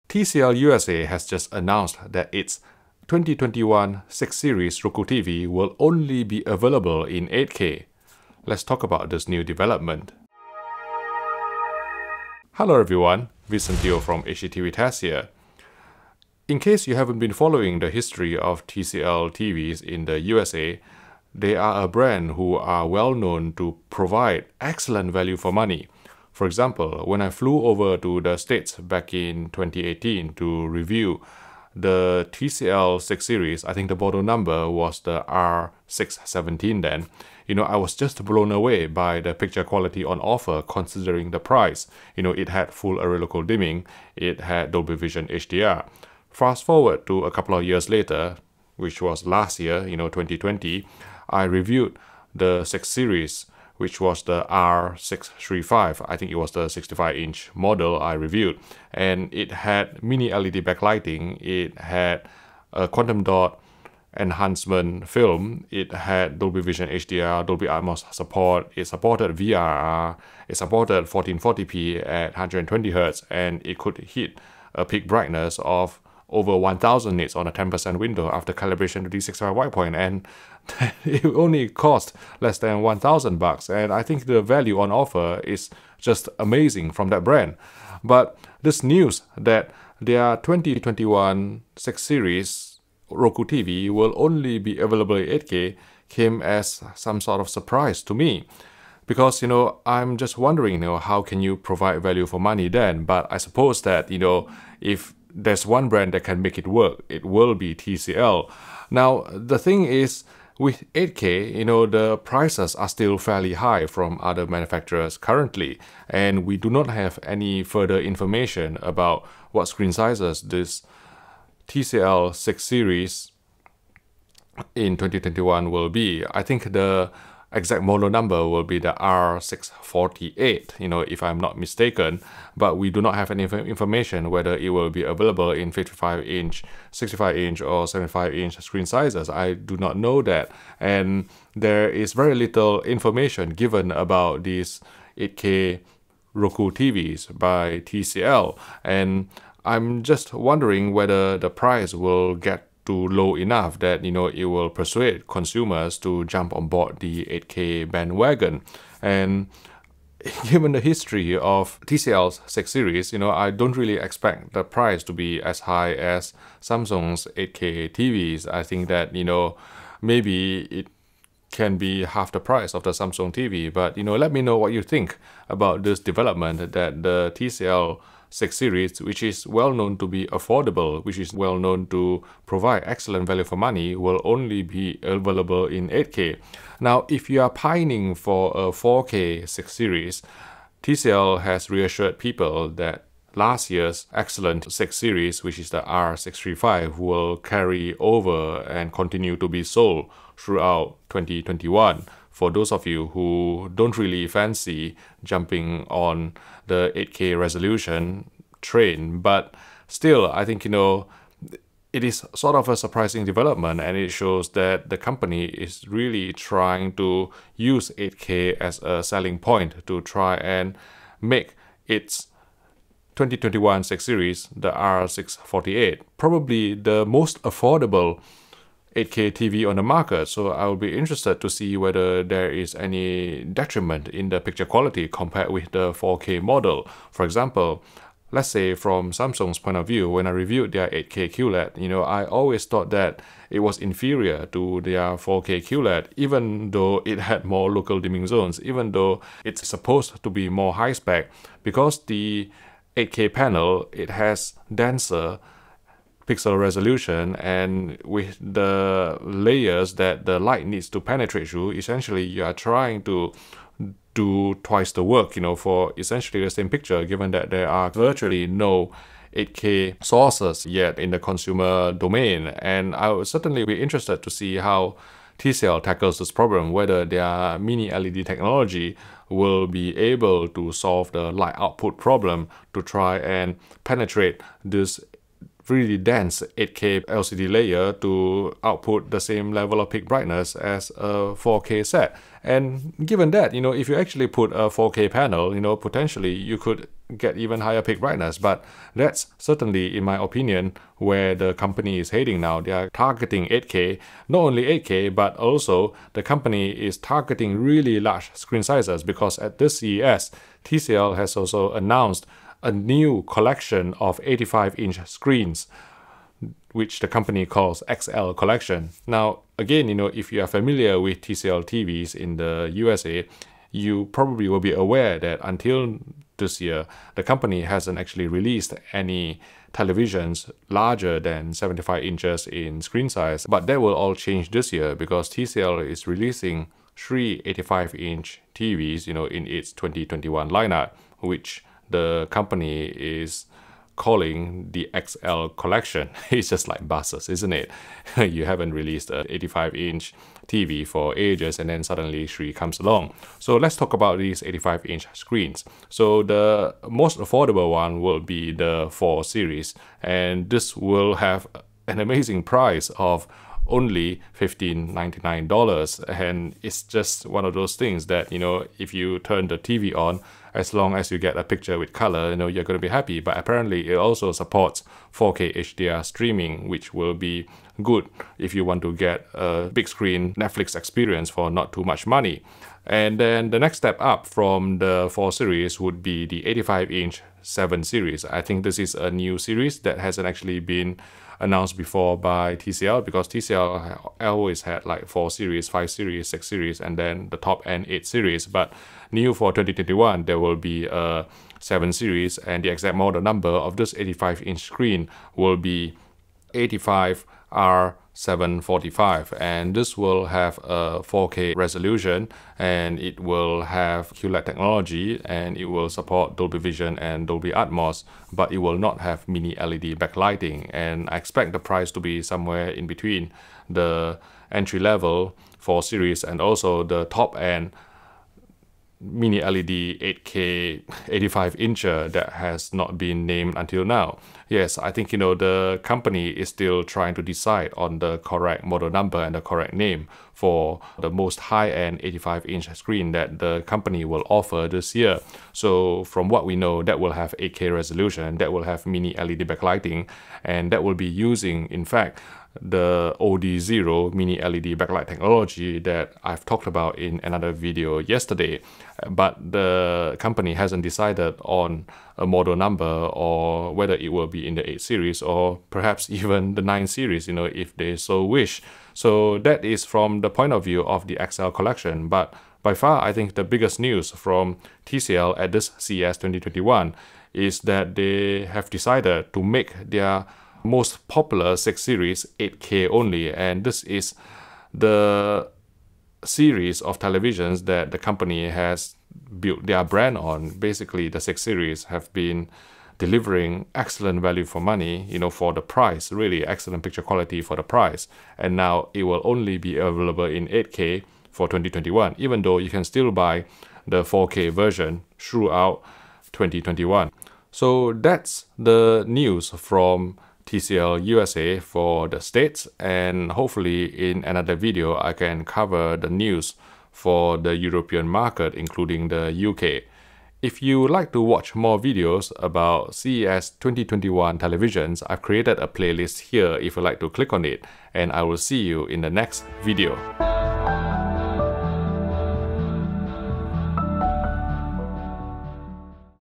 TCL 2021 6-Series Mini LED TV is 8K Only - The Beginning of The End for 4K?
TCL has revealed that its 2021 6-Series Roku TV with Mini LED backlight technology will only be available in 8K resolution. Is this the beginning of the end for 4K?

The company also unveiled a trio of 85-inch TVs known as the TCL XL Collection. There's the entry-level 4-Series priced at only $1599, then the 7-Series (model number 85R745) with 4K resolution, full-array local dimming, QLED colours, Dolby Vision & Atmos; and an unnamed top-end 85-inch 8K TV with OD Zero MiniLED backlight technology.